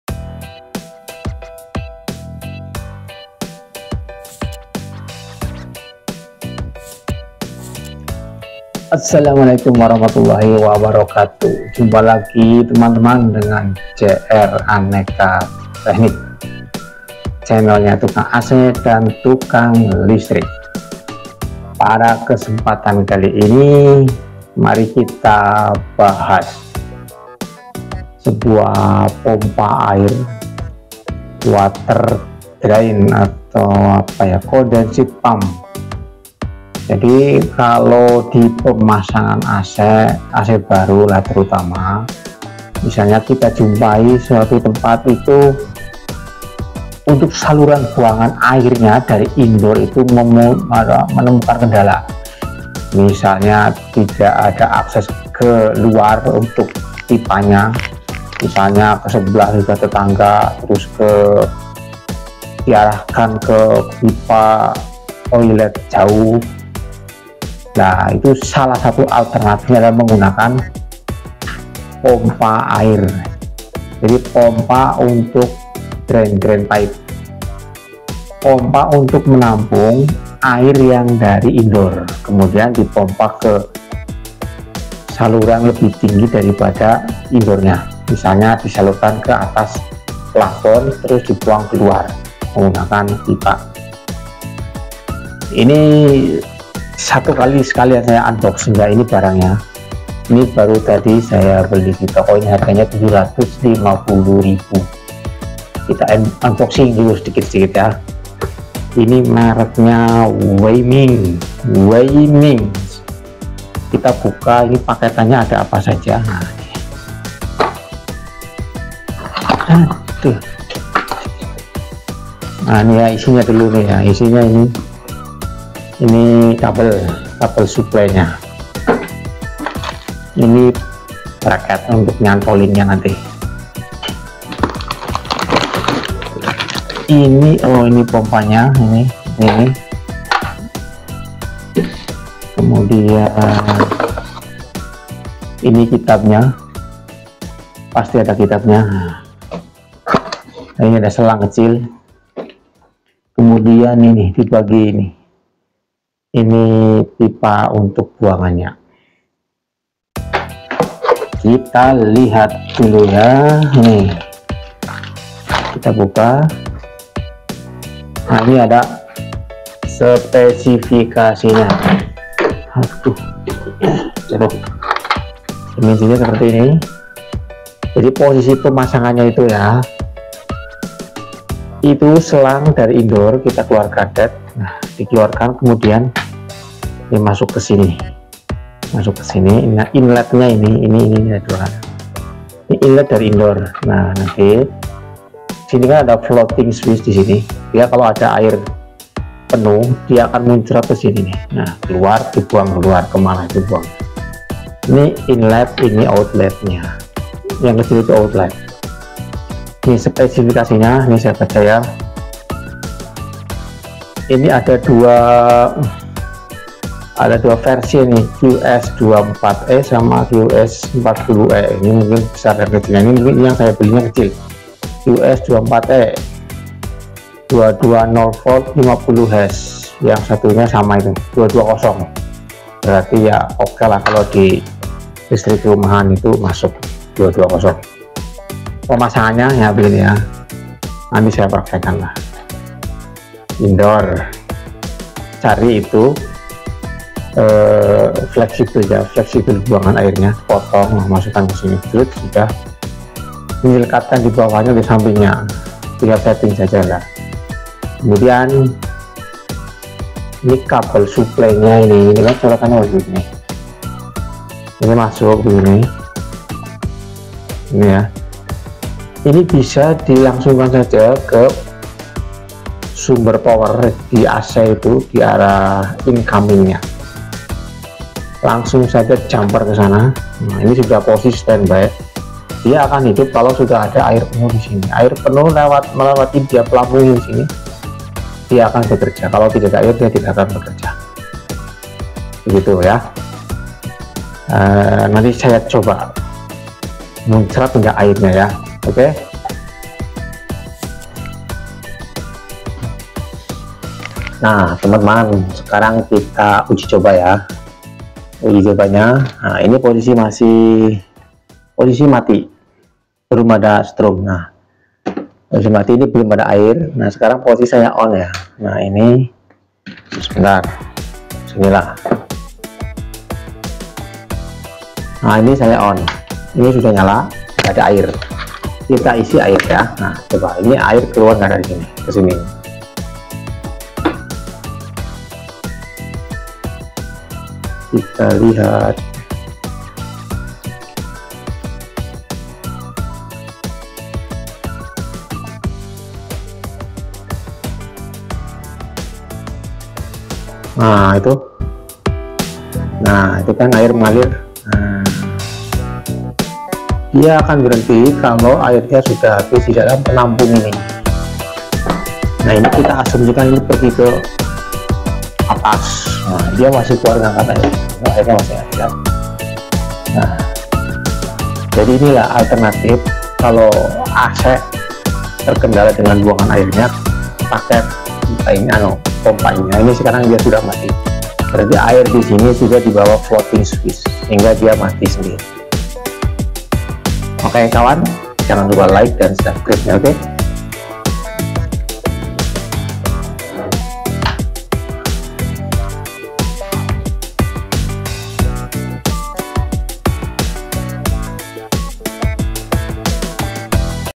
Condensate Pump Pompa Alternatif buangan Air AC
Salam Segar,Salam dingin bersama@JR Aneka Teknik,Divideo ini Saya uraikan pengaplikasian CONDESATE PUMP/Pompa air mini untuk memopa buangan air ac split jika kesulitan  membuat saluran air karena lokasi ,kondisi dan spek yang terbatas.DEngan CONDENSATE PUMP air bisa kita alirkan kebagian yang lebih tinggi dari indoor,seperti dialirkan diatas plafon kemuadian dibuang ke luar ruangan/saluran air.